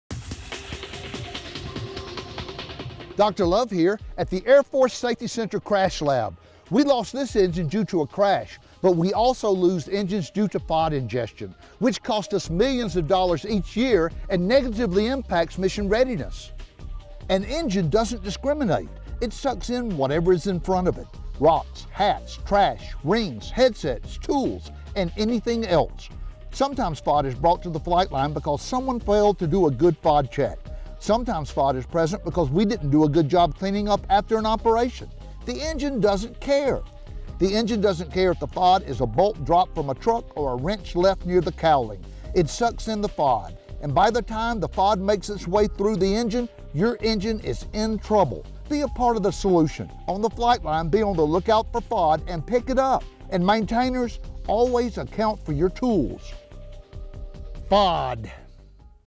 Foreign Objects Cause Damage
The Air Force has experienced just over 300 Class A-C mishaps related to foreign object damage since fiscal year 2014. Foreign objects are a substance, debris or article alien to a vehicle or system which can cause significant damage. In an effort to combat the issue, Dr. Love takes a moment to highlight why it is so important for Airmen to stay vigilant when checking their vehicles prior to entering the flightline and accounting for their equipment while working on aircraft.